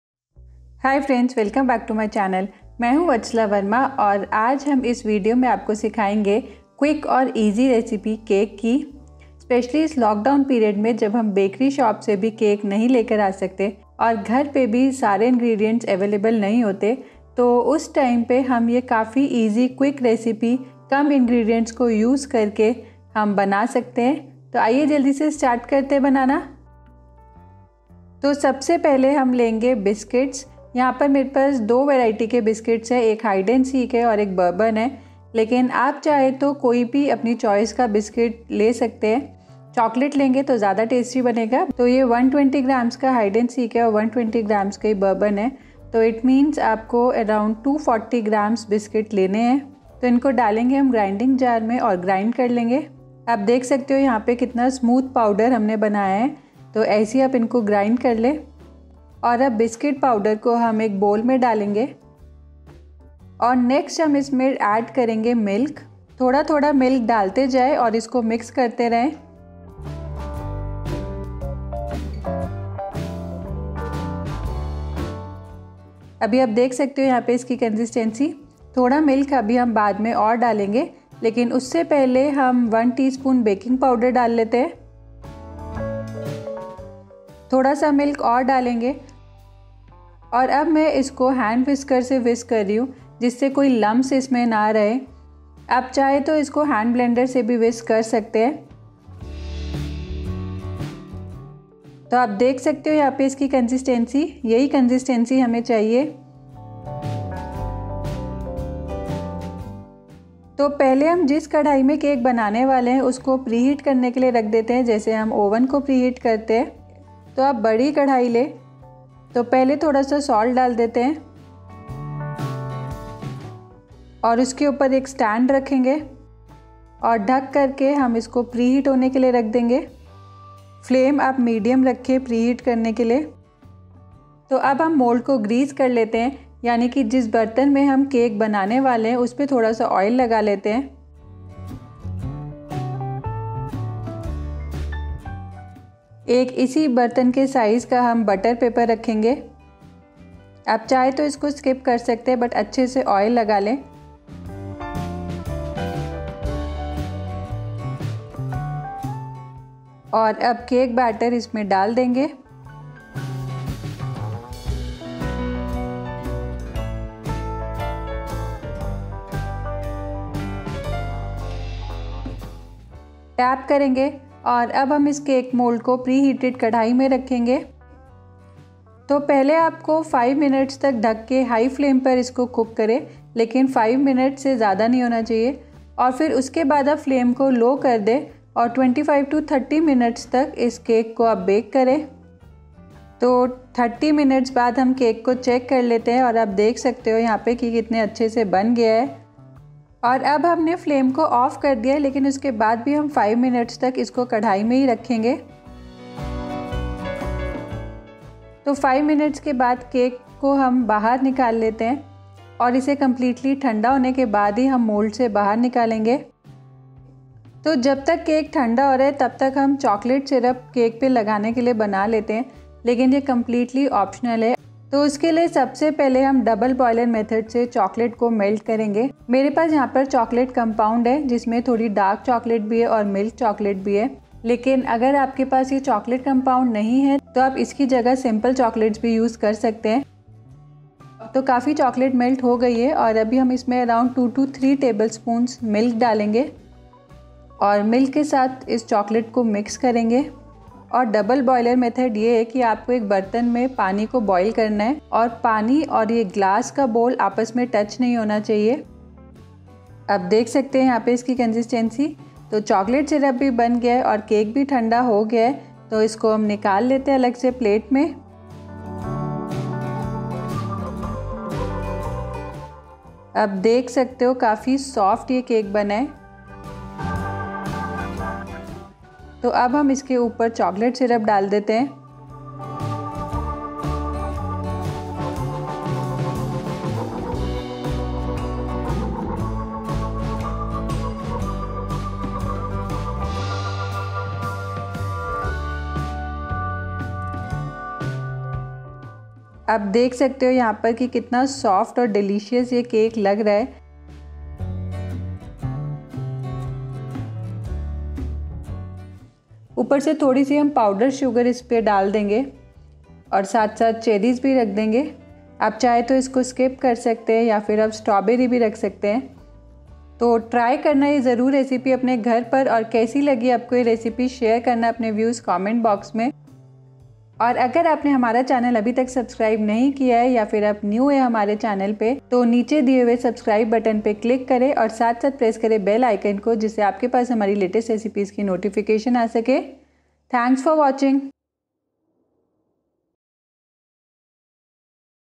3 Ingredient CHOCOLATE CAKE in Kadai ! Lock-down recipe | without Egg, Oven, Maida
3 Ingredient CHOCOLATE CAKE in Kadhai !  Lock-down recipe | without Egg, Oven, Maida

3 ingredient lock down Chocolate Cake ( in Kadhai):

-  250 grams Chocolate Cookies ( 3 cups, measure the cookies once crushed into a fine powder )
-  Milk (3/4 Glass)
-  1 tsp Baking Powder

*PLEASE READ* : The flavor of your cake will depend on what cookies you choose to use! For more Chocolate flavor you can add:
- melted Chocolate mixed with milk
- Chocolate Chips
- cherry/strawberry /icing sugar